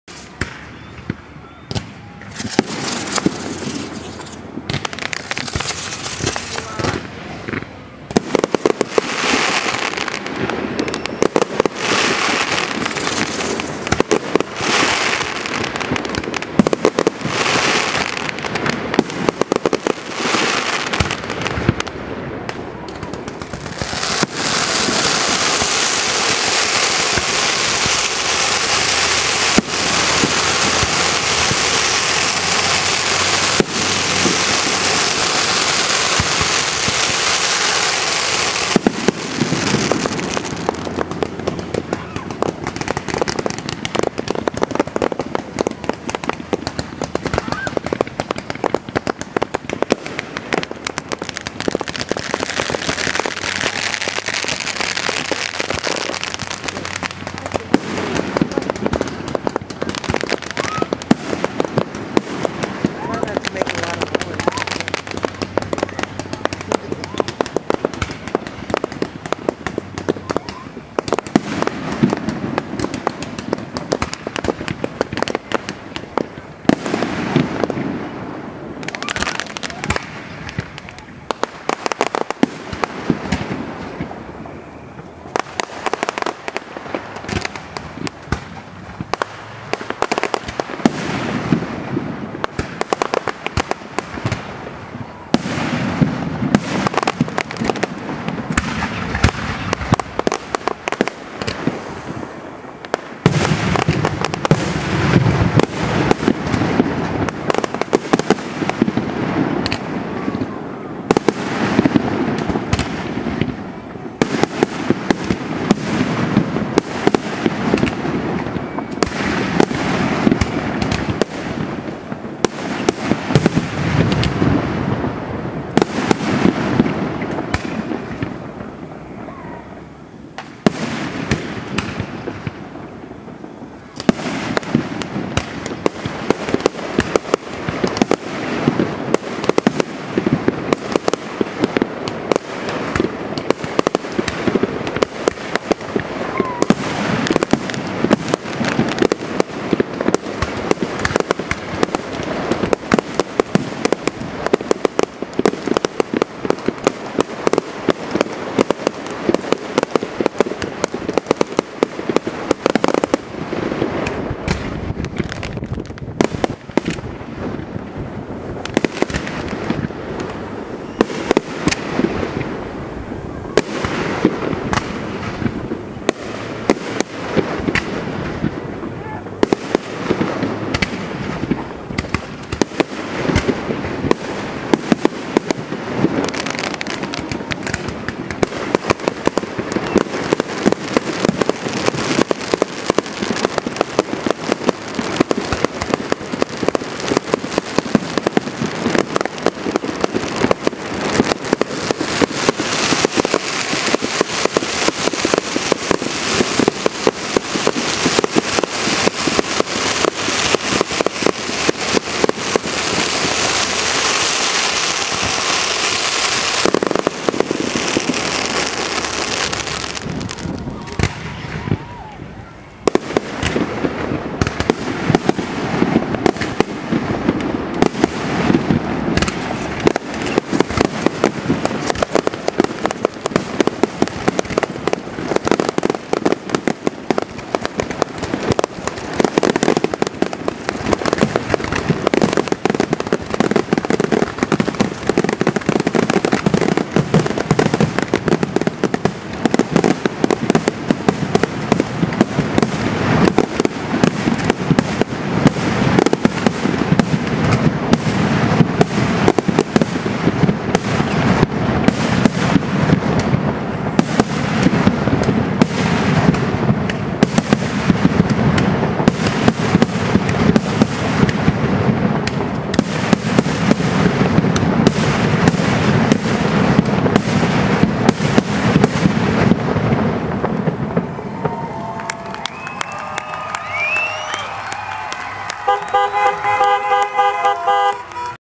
Mexico Missouri Fireworks from Garfield Park 2009 Part 2
Fourth of July fireworks shot off at Garfield Park in Mexico Missouri.